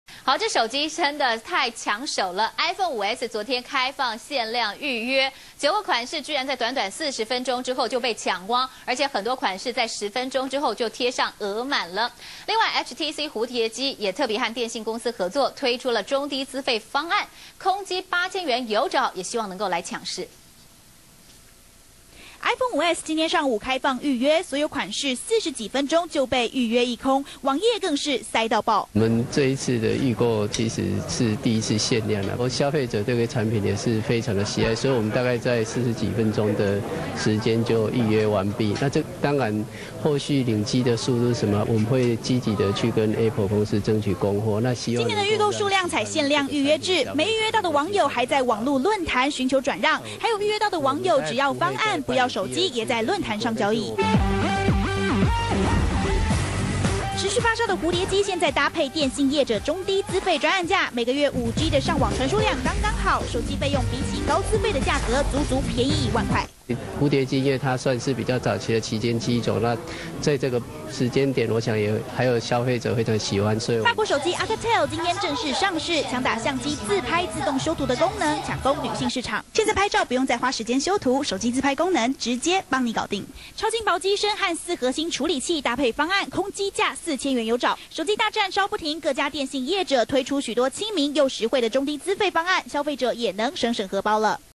中視新聞》iPhone 5S預購 9款式40分鐘全搶光
內容轉載自《中時電子報》：
中視新聞》iPhone 5S開放限量預約，9個款式短短40分鐘就被搶光，很多十分鐘就貼額滿。而hTC蝴蝶機也和電信公司合作，推出中低資費專案，空機八千元就有找。